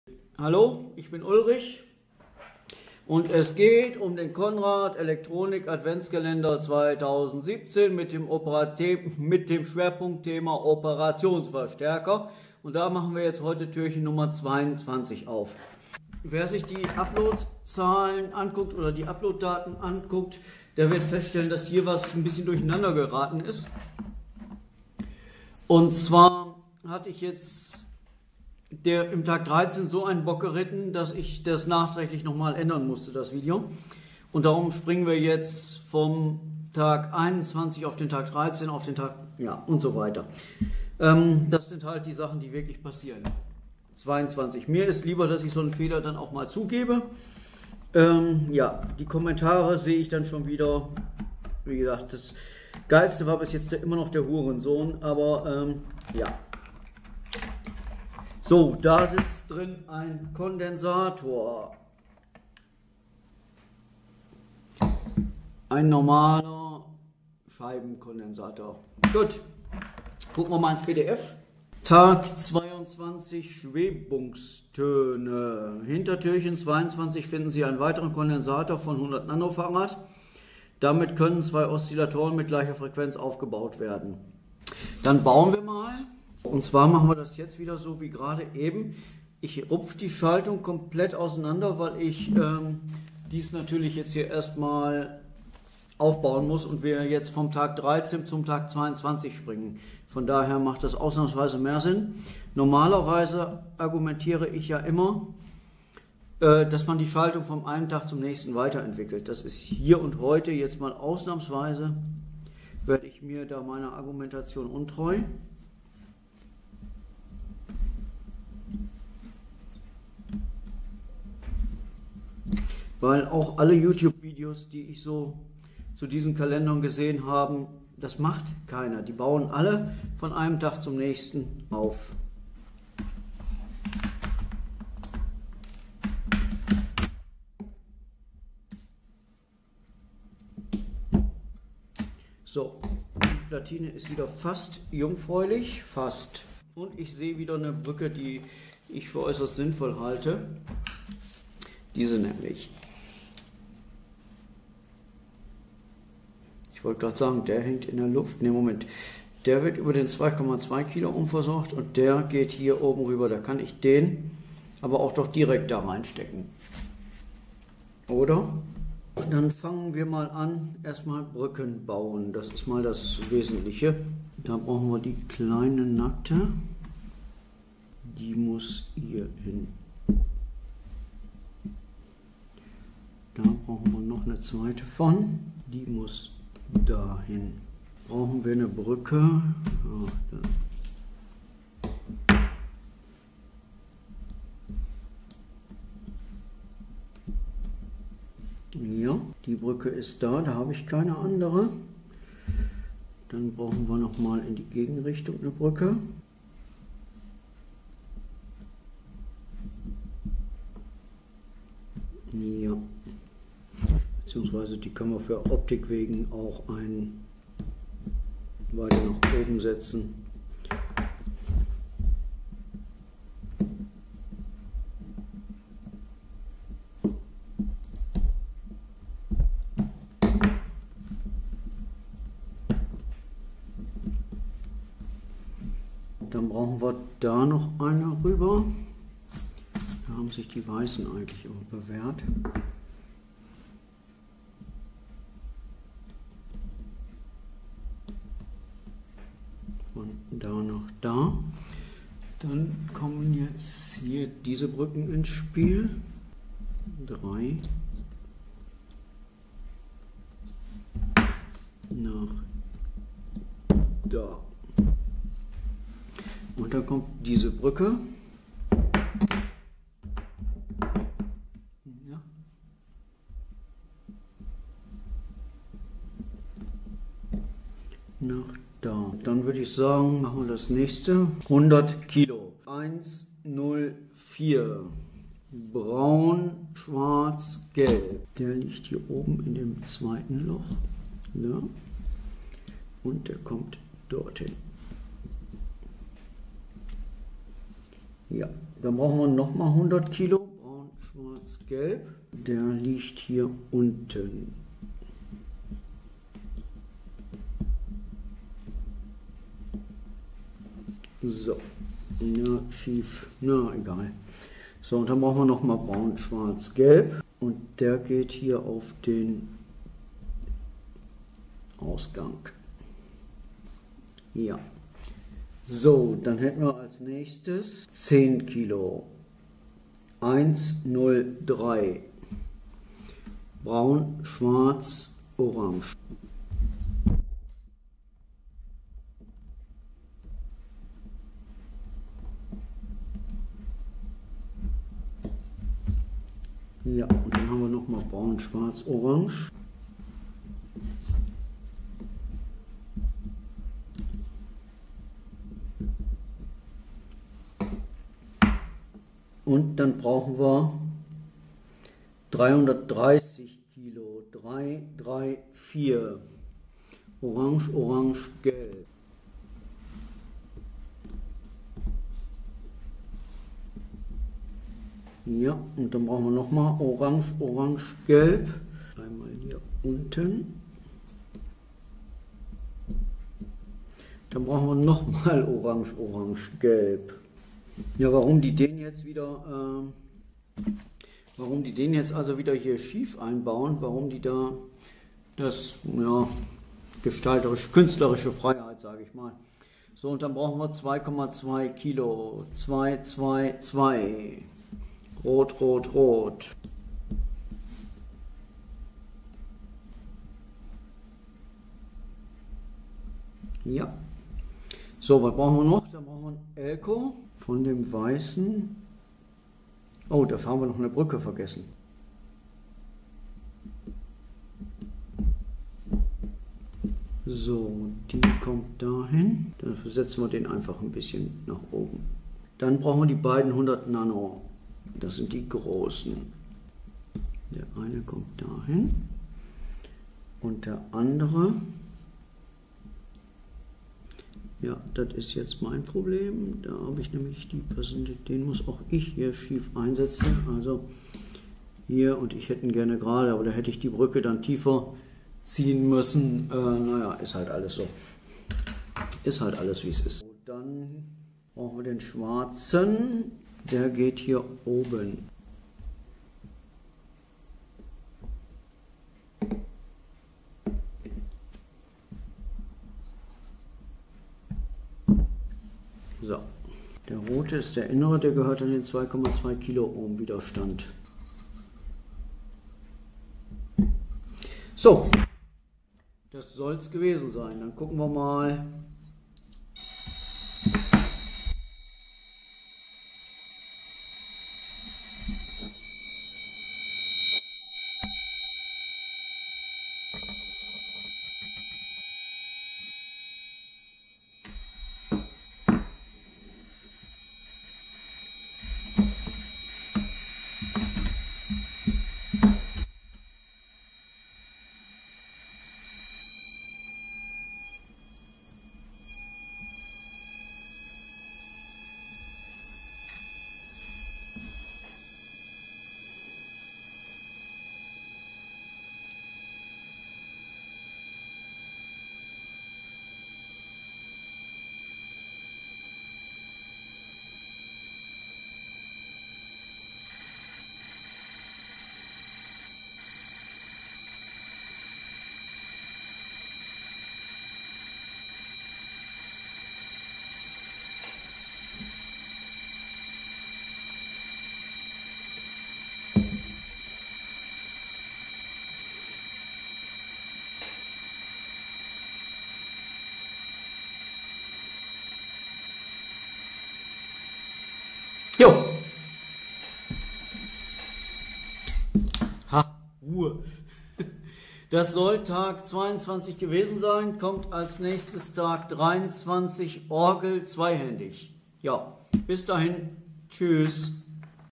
Tag 22; 2017; Conrad Elektronik Adventskalender; Schwebungstöne
Aufbau des Experimentes vor laufender Kamera